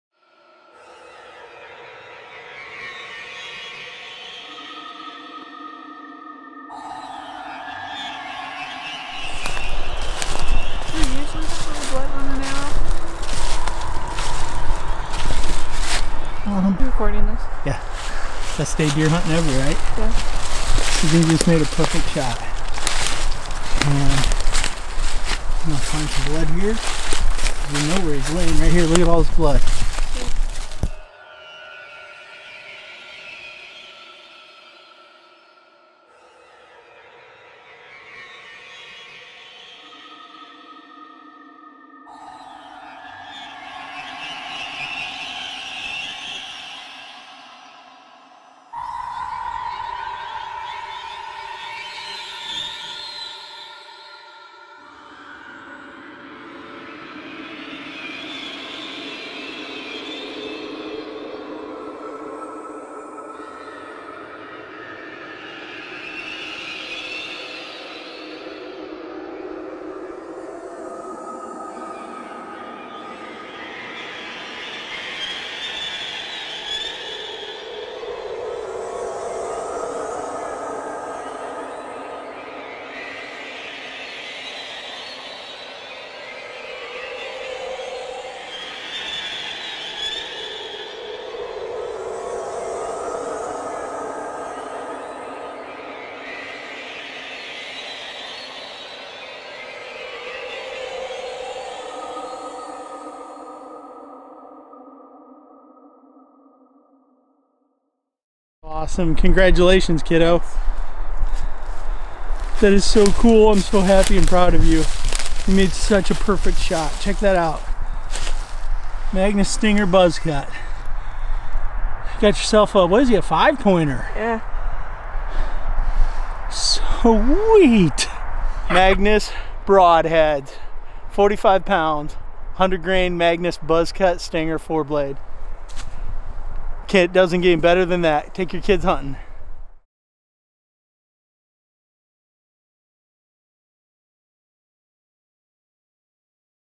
Magnus Broadheads - Blood Trails Edition - Sydney Craig Indiana Buck
The Magnus Black Stinger Buzzcut is a great option for low poundage setups and creates excellent blood trails.  Sydney is shooting 45# and 26 1/2" draw.  She slid this arrow through this deer before he even knew what hit him.  This is her 3rd complete pass through with a Magnus Buzzcut at low poundage.  A huge Thank You goes out to Derek Craig for sending us this footage.